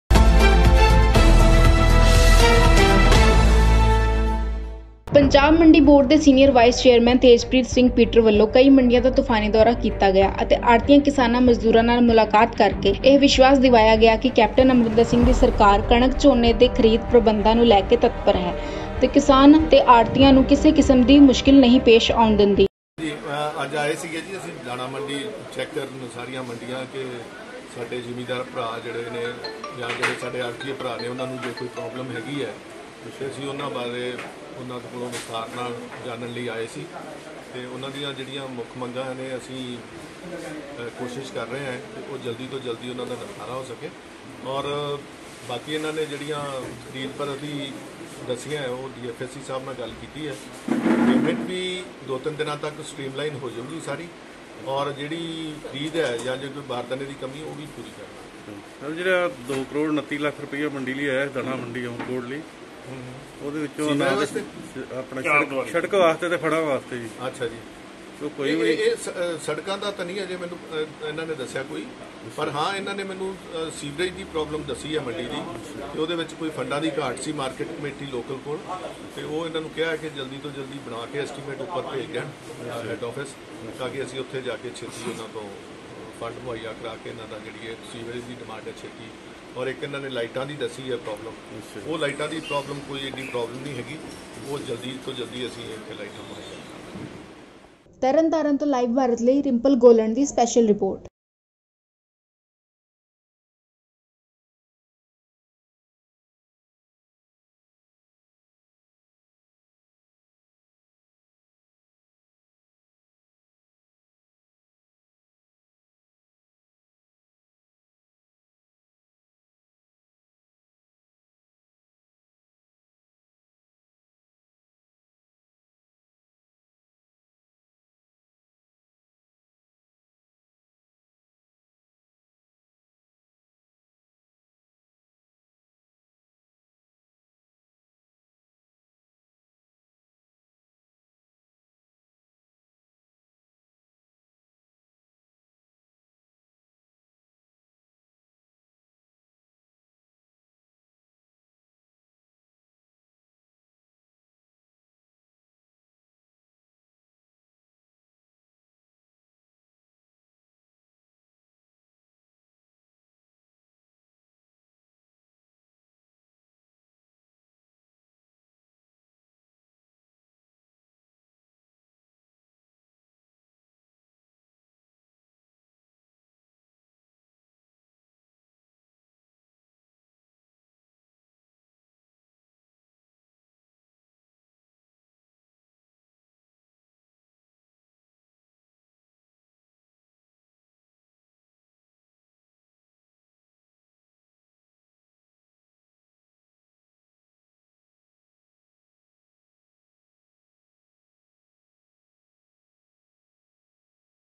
ਪੰਜਾਬ ਮੰਡੀ ਬੋਰਡ ਦੇ ਸੀਨੀਅਰ ਵਾਈਸ ਚੇਅਰਮੈਨ ਤੇਜਪ੍ਰੀਤ ਸਿੰਘ ਪੀਟਰ ਵੱਲੋਂ ਮੰਡੀਆਂ ਦਾ ਤੂਫ਼ਾਨੀ ਦੌਰਾ
latest news
punjab news